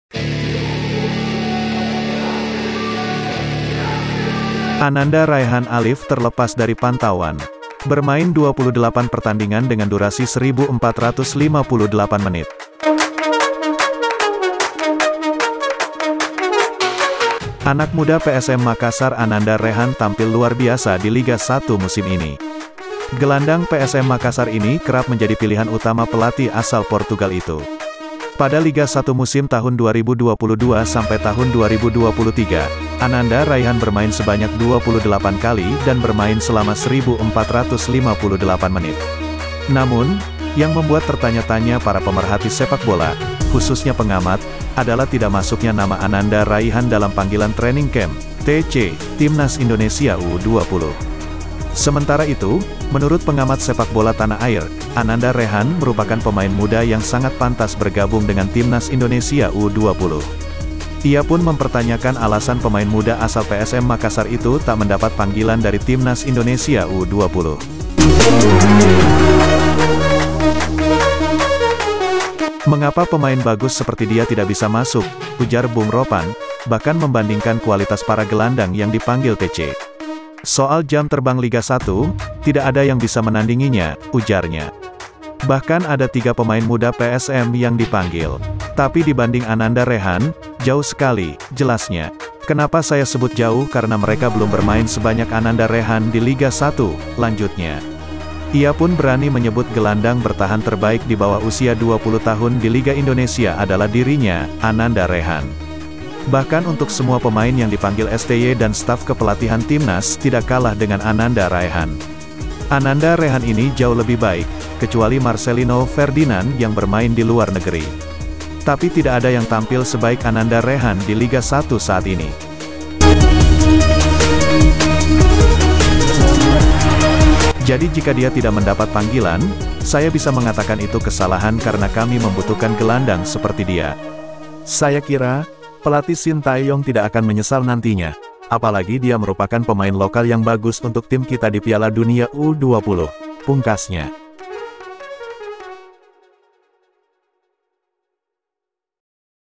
STY Blunder !!! Ananda Raehan terlupakan malah pilih pemain jarang bermain
Ananda Raehan Alif terlepas dari Pantauan .
Bermain 28 Pertandingan dengan durasi 1458 Menit.

Anak muda PSM Makassar Ananda Rehan tampil luar biasa di Liga 1 musim ini. Gelandang PSM Makassar ini kerap menjadi pilihan utama pelatih asal Portugal itu. Pada Liga 1 musim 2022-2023, Ananda Raihan bermain sebanyak 28 kali dan bermain selama 1458 menit.
Namun, yang membuat tertanya -tanya  para pemerhati sepak bola, khususnya pengamat, adalah tidak masuknya nama Ananda Raihan dalam panggilan Training Camp (TC) Timnas Indonesia U-20.
Sementara itu, menurut pengamat sepak bola tanah air, Ananda Rehan merupakan pemain muda yang sangat pantas bergabung dengan Timnas Indonesia U-20. Ia pun mempertanyakan alasan pemain muda asal PSM Makassar itu tak mendapat panggilan dari timnas Indonesia u 20.
Mengapa pemain bagus seperti dia tidak bisa masuk?" tambahnya. Bung Ropan, bahkan membandingkan kualitas para gelandang yang dipanggil TC. Soal jam terbang Liga 1, tidak ada yang bisa menandinginya, ujarnya.
Bahkan ada tiga pemain muda PSM yang dipanggil. Tapi dibanding Ananda Rehan, jauh sekali," jelasnya. "Kenapa saya sebut jauh karena mereka belum bermain sebanyak Ananda Rehan di Liga 1," lanjutnya.
 Ia pun berani menyebut gelandang bertahan terbaik di bawah usia 20 tahun di Liga Indonesia adalah dirinya, Ananda Rehan. Bahkan untuk semua pemain yang dipanggil STY dan staf kepelatihan Timnas tdk kalah dengan Ananda Raehan.
Ananda Rehan ini jauh lebih baik, Kecuali Marcelino Ferdinand yang bermain di luar negeri. "Tapi tidak ada yang tampil sebaik Ananda Rehan di Liga 1 saat ini.
Jadi jika dia tidak mendapat panggilan, saya bisa mengatakan itu kesalahan karena kami membutuhkan gelandang seperti dia. Saya kira,Pelatih Shin Tae young tidak akan menyesal nantinya, apalagi dia merupakan  pemain-pemain lokal yang bagus untuk tim kita di Piala Dunia U-20,” pungkasnya.